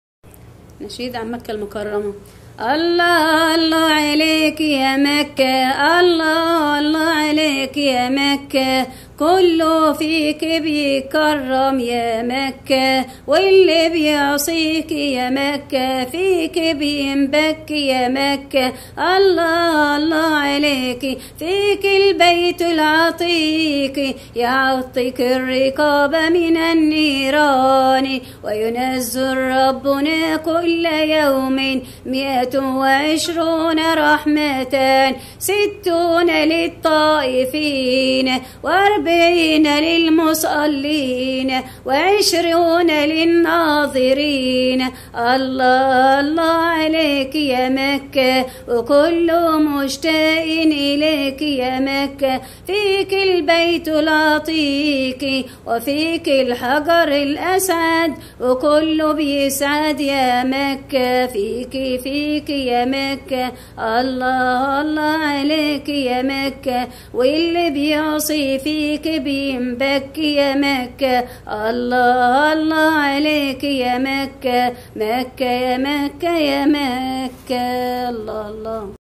نشيد عن مكة المكرمة للأستاذة الفاضلة بطة عطيه ماما حبيبتي وبابا حبيبي والدكتور كمال حبيبنا ربنا
اللهم أجبر خاطر والدى ترتيل القرآن الكريم للأستاذة الفاضلة بطة عطيه ماما حبيبتي وبابا حبيبي والدكتور كمال حبيبنا ربنا يكرمكم ويسعدكم في الدنيا والاخرة